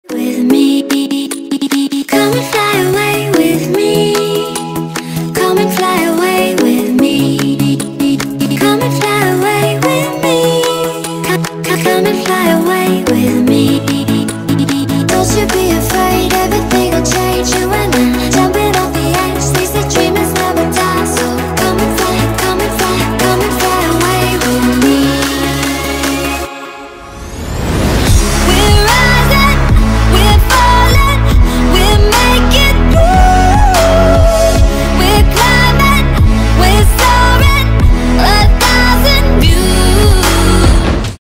•Fly Away For Me• || STH || Sarah the Hedgehog || Shadrah ❤️💜
Shadow has a surprise for his dearly beloved Sarah.

Characters:
Shadow the Hedgehog
Sarah the Hedgehog/Sarah Robotnik (OC)